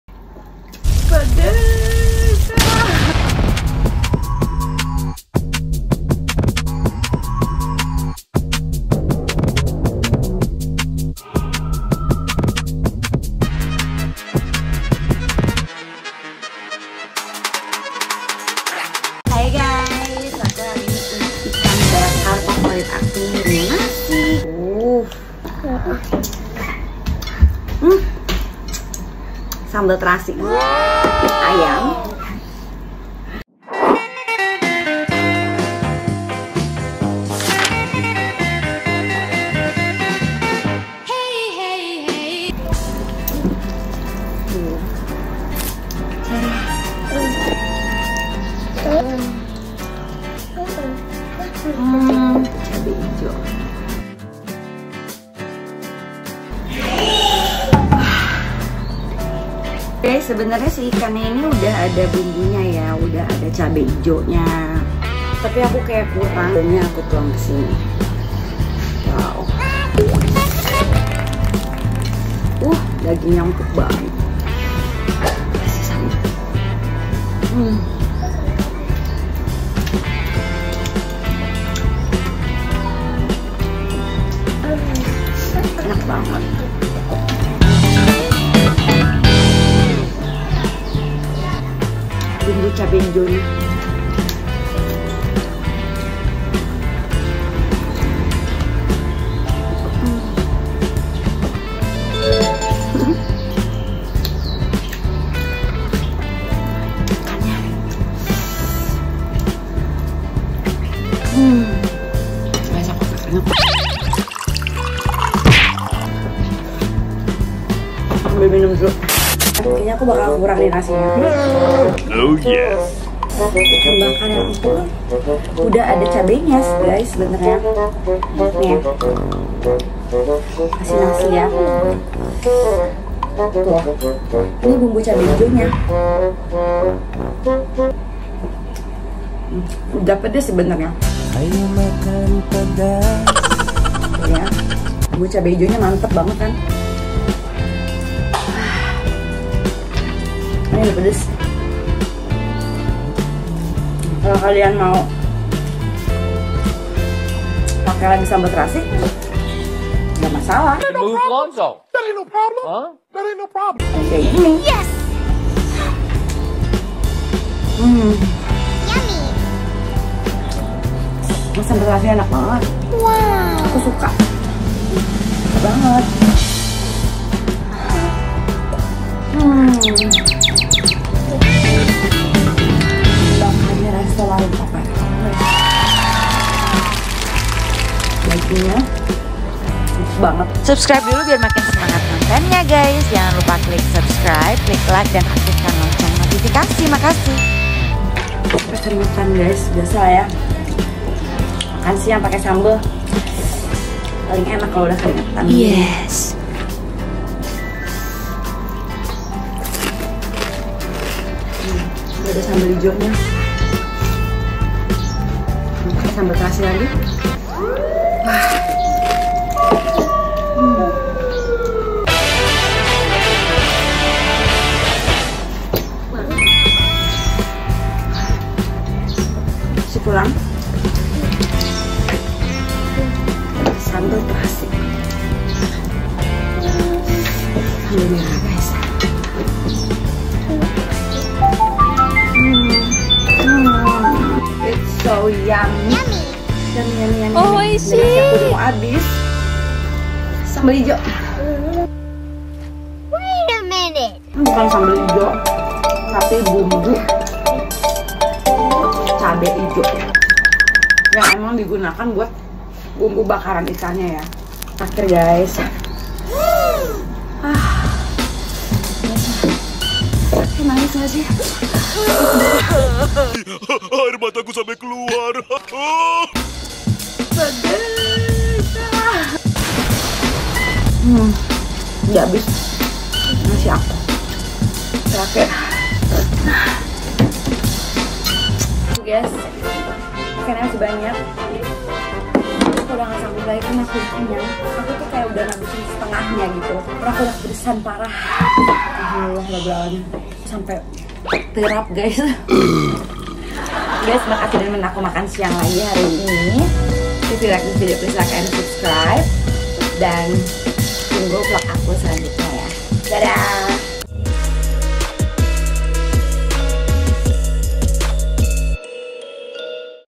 Mukbang: Ikan Kuwe Bakar Cabe Ijo
Hai guys! 🌶️ Kali ini kita nyobain ikan kuwe bakar cabe ijo dan ayam bakar sambal terasi dari resto warung papatong. Double kill pedasnya! 🔥🔥

Makasih resto warung papatong pakansari cibinong buat supportnya! Selamat makan! 🍽️🔥

Dukungan & Apresiasi
Terima kasih atas dukungan dari kalian melalui BCA: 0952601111 Nike Roshana